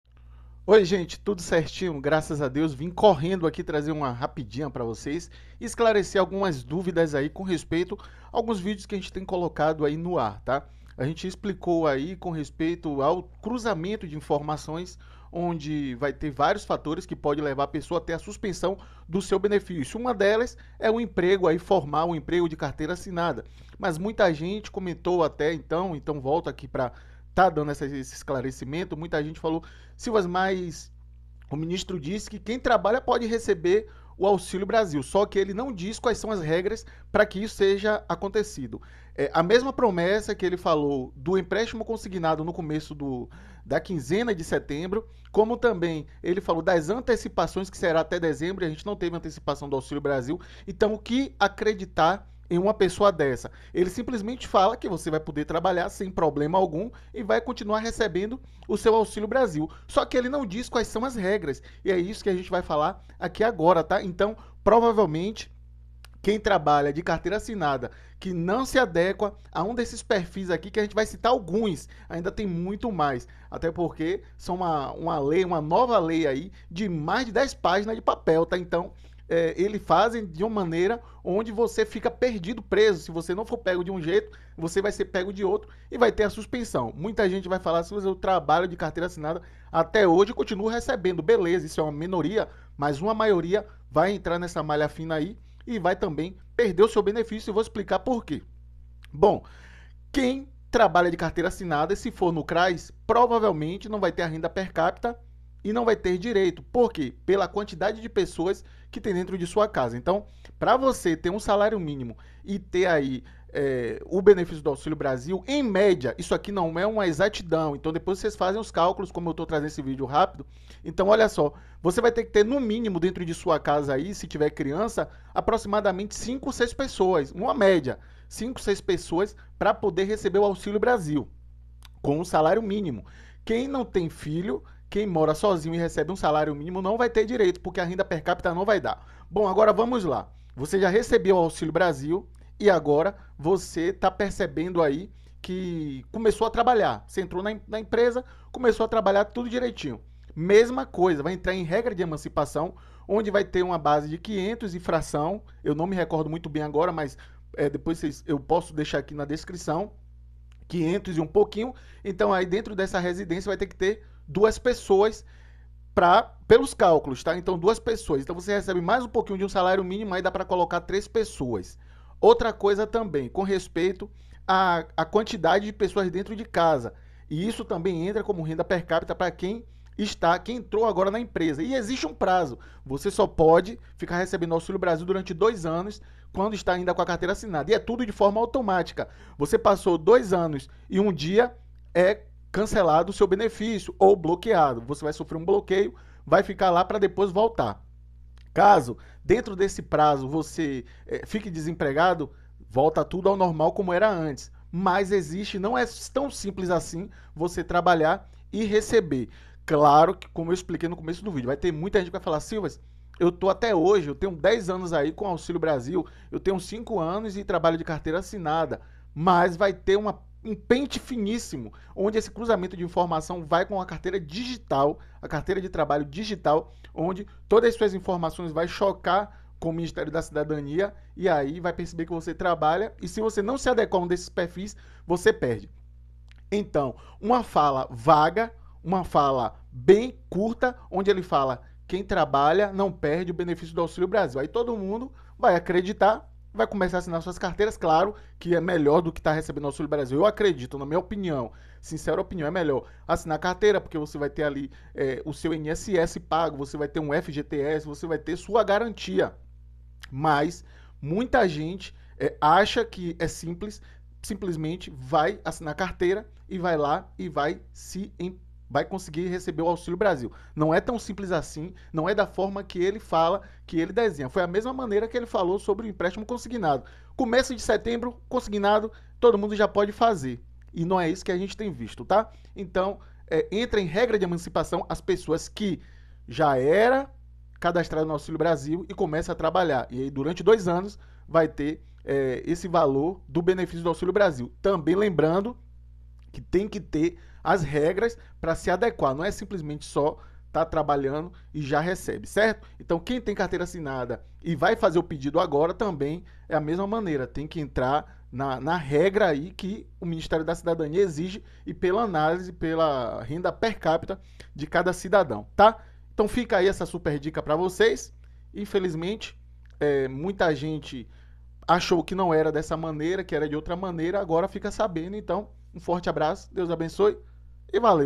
AUXILIO BRASIL QUEM TEM CARTEIRA ASSINADA PODE PERDER
Quem tem carteira Assinada pode perder o Auxilio Brasil entenda